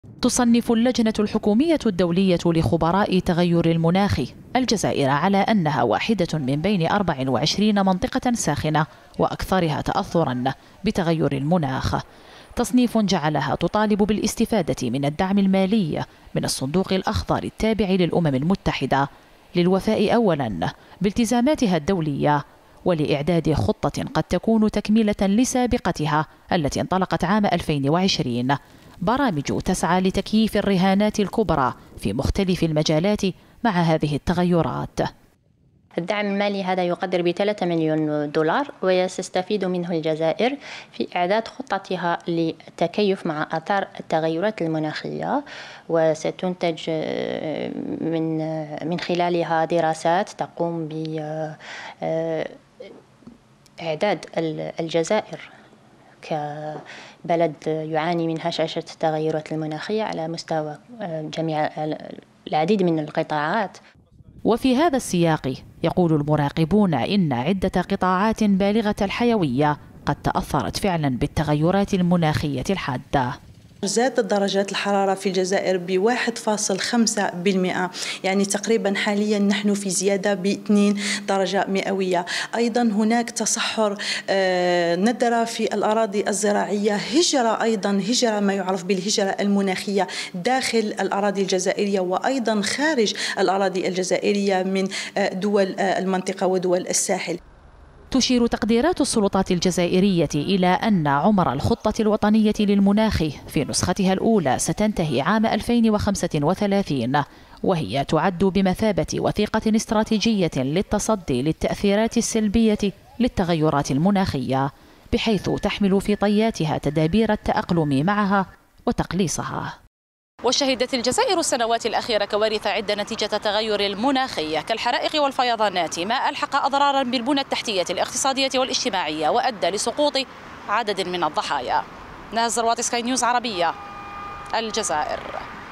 الجزائر تحصل على دعم يقدر بـ3 ملايين دولار لمحاربة التغيرات المناخية
أعلنت الجزائر أنها حصلت على ثلاثة ملايين دولار من الصندوق الأخضر التابع للأمم المتحدة بهدف محاربة التغير المناخي.
وتتسبب هذه الظاهرة سنويا بخسائر مادية وبشرية للبلاد، فيما تسعى السلطات لوضع خطط وطنية للتقليص من آثارها.